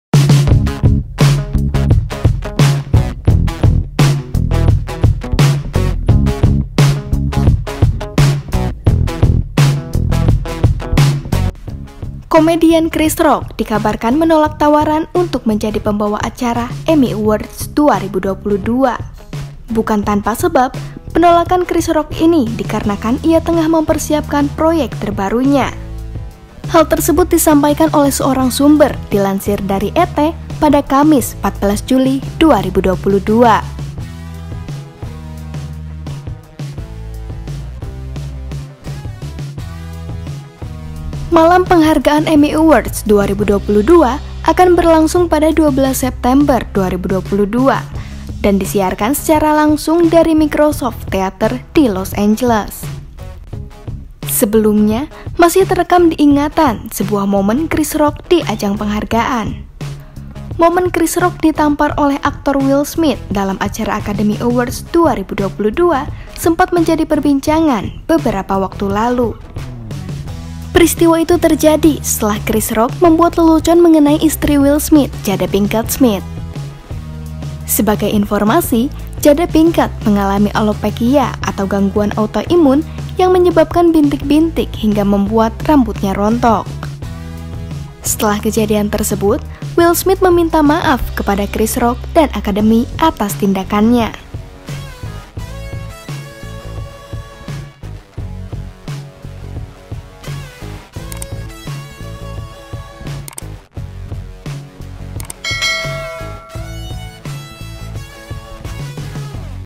Chris Rock Tolak Tawaran Pandu Acara Ajang Penghargaan, Ini Alasannya
Komedian Chris Rock dikabarkan menolak tawaran untuk menjadi pembawa acara Emmy Awards 2022.
Penolakan Chris Rock ini dikarenakan ia tengah mempersiapkan proyek terbarunya.

Hal tersebut disampaikan oleh seorang sumber dilansir dari ET pada Kamis (14/7/2022).

Malam penghargaan Emmy Awards 2022 akan berlangsung pada 12 September 2022 dan disiarkan secara langsung dari Microsoft Theater di Los Angeles.

Penulis: Baharudin Al Farisi
Penulis Naskah: Fathira Deiza A
Narator: Fathira Deiza A
Video Editor: Fathira Deiza A
Produser: Ira Gita Natalia Sembiring